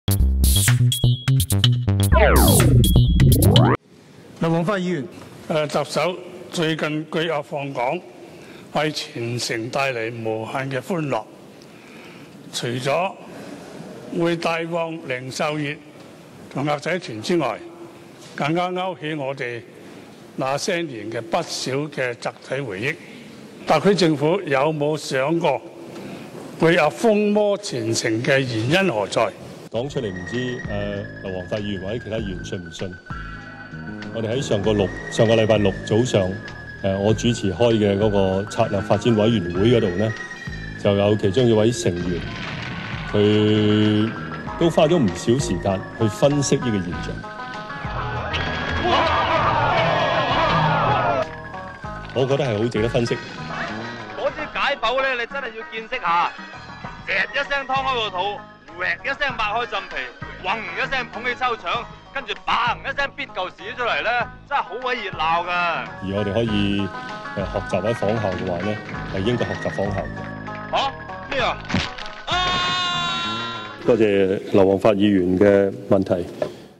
香港策略發展 還看梁粉解剖膠鴨
為香港宏觀經濟發展出謀獻策的「策略發展委員會」，非常緊貼香港的最新情況；據策發會主席、特首梁振英透露，他們在上週才花了很長的時間，討論黃色巨鴨瘋靡香港的現象，抽絲剝繭詳細分析.